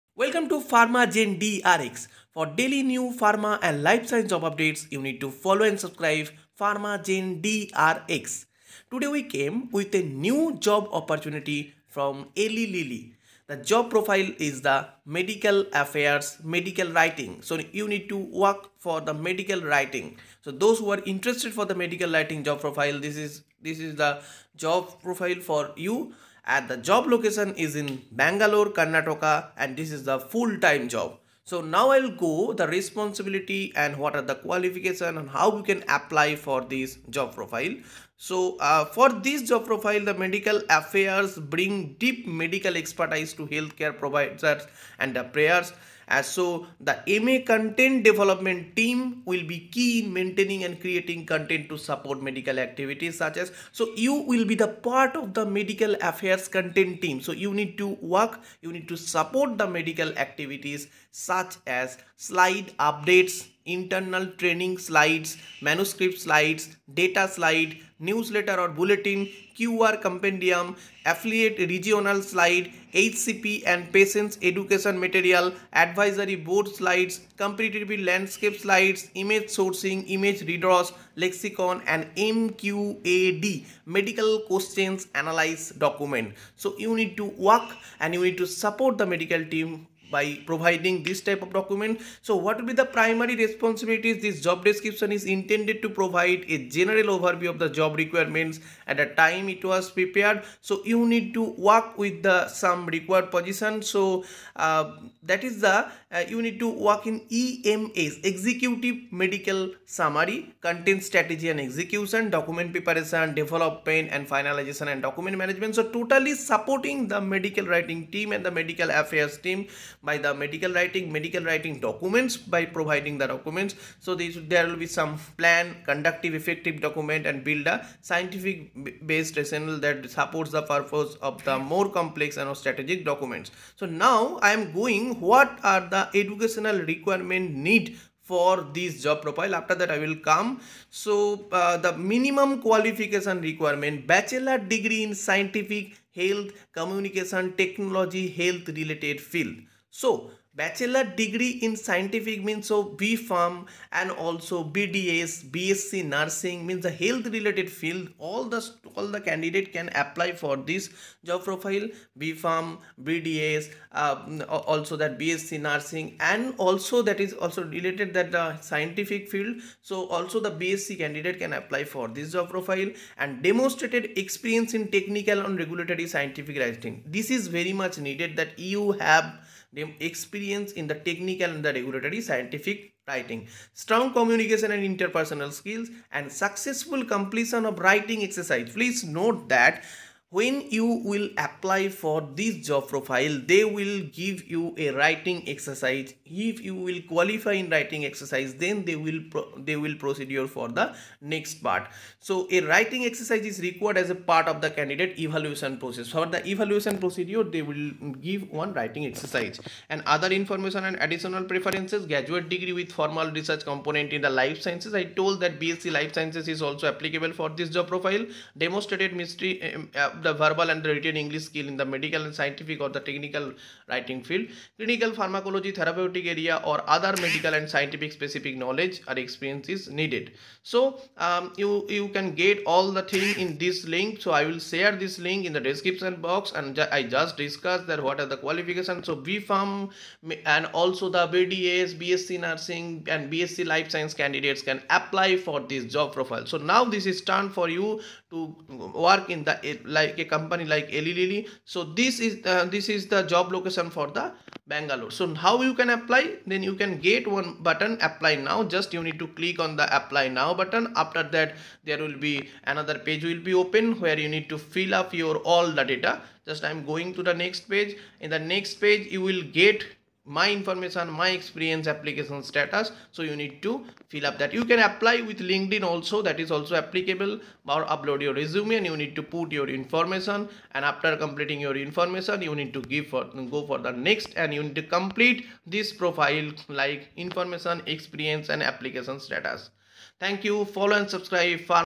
Eli Lilly Medical Writing Job | Pharmacy Job
Eli Lilly  is hiring for Medical Affairs – Medical Writing

Job Role : Medical Affairs – Medical Writing

Job Location : Bangalore

Qualifications :

Bachelor’s degree in a scientific, health, communications, technology health related field.

Demonstrated experience in technical/ regulatory scientific writing.

Strong communication and interpersonal skills.